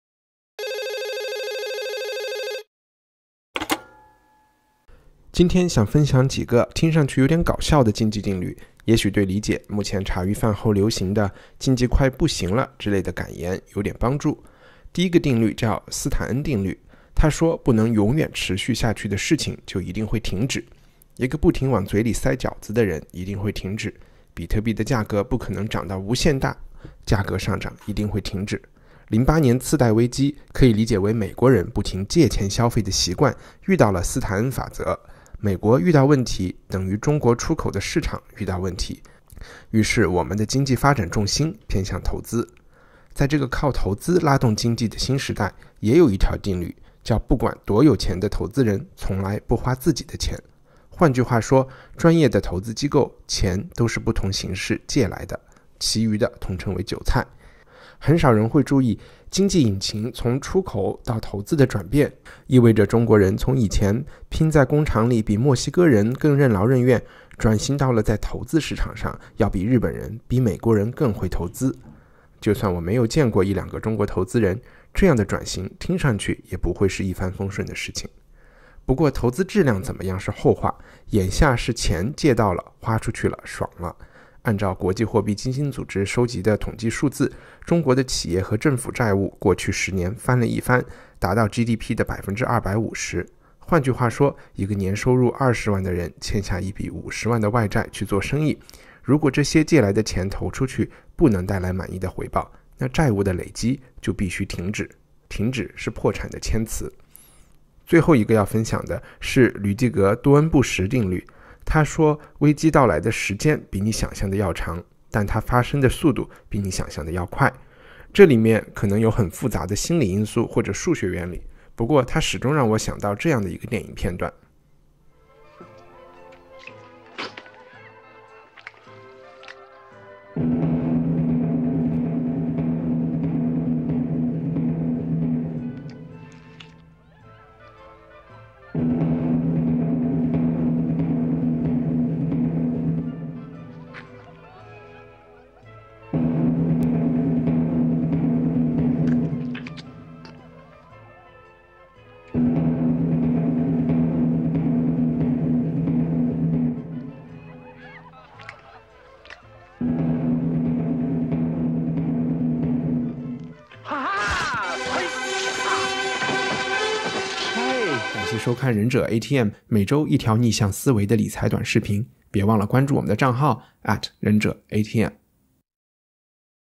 三个搞笑的市场定律，帮大盘续命1000点
今天想分享几个听上去有点搞笑的经济定律，也许对理解2008年金融危机后中国经济转型和现在遇到的一些问题有点帮助。